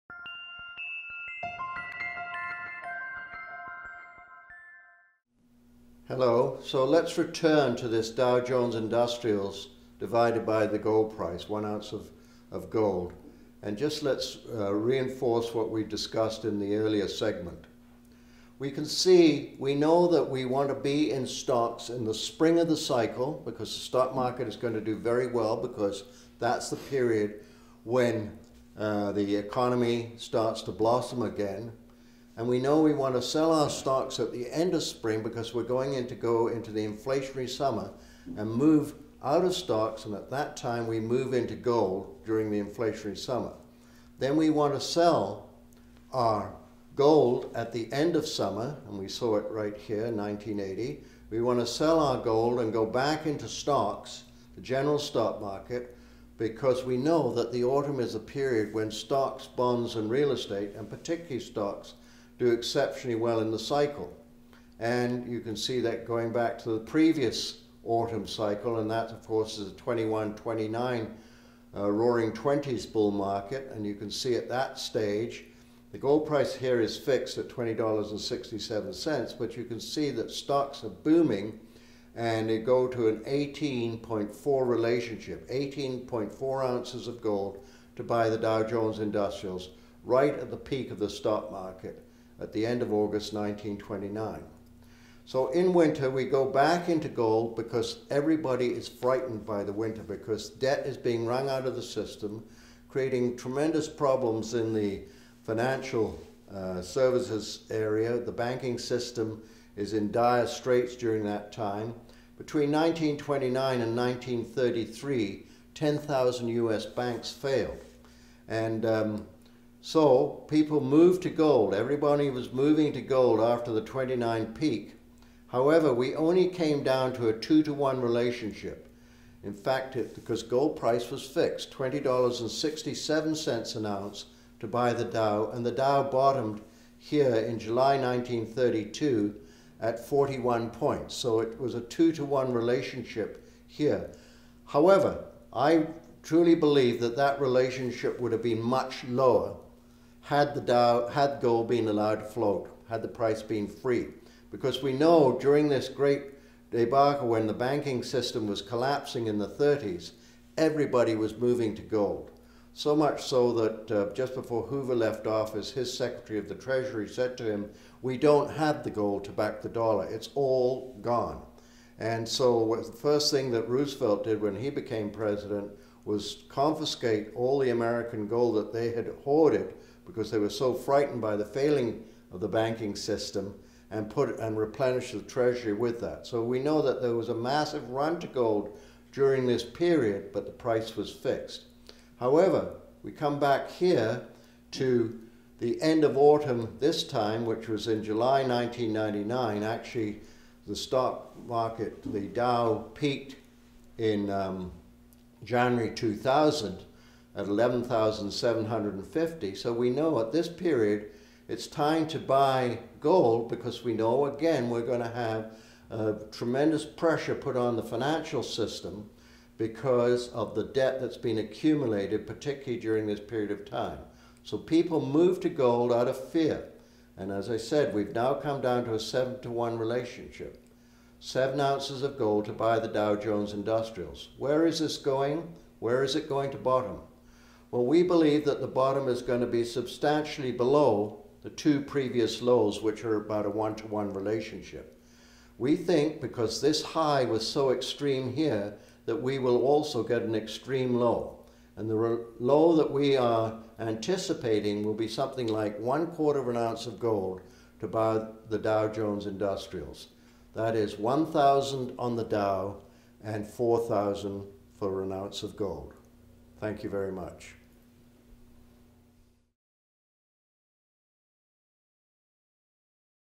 Dow Vs Gold Ratio Chapter 02
Dow vs Gold Ratio

Longwave Analytics Economic Forecaster Ian Gordon explains the inverse gold bullion price / Dow Jones Industrial Average point level relationship, in terms of their historical peaks and troughs, coincident with the seasonality of the long wave economic cycles.  Pivotal trading points are identified where an investor should sell all the stocks in his/her portfolio and buy gold bullion and/or gold stocks; as well as vice versa.